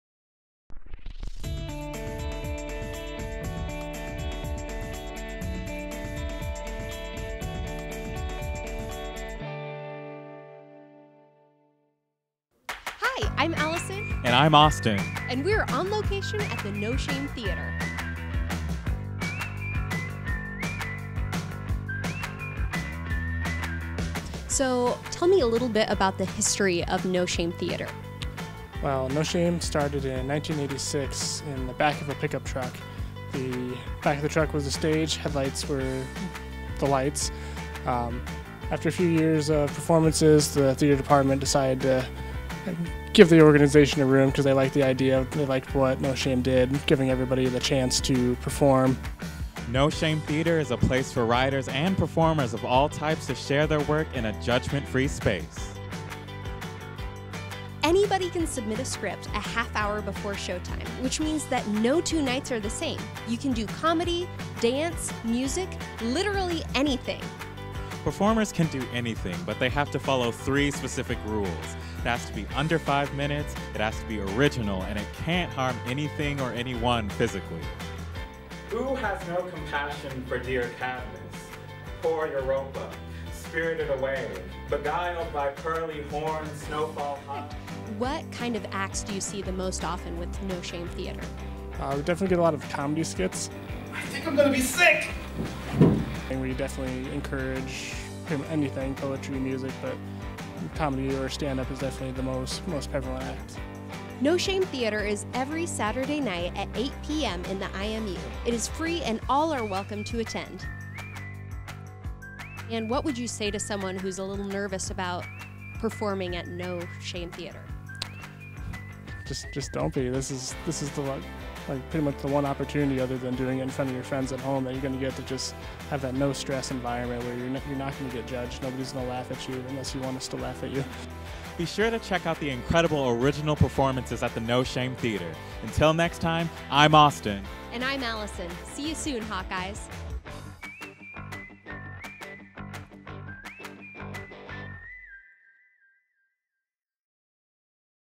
On Location: No Shame Theater
Austin and Allison are headed to the No Shame Theater, where anyone and everyone can submit an original script in front of a judgment-free audience. The only rules: pieces must be original, under 5 minutes, and not damage attendees or equipment. Other than that, anything goes!

Visit Iowa

Apply

Accepted?

If you have been admitted, accept through your admissions profile at myui.uiowa.edu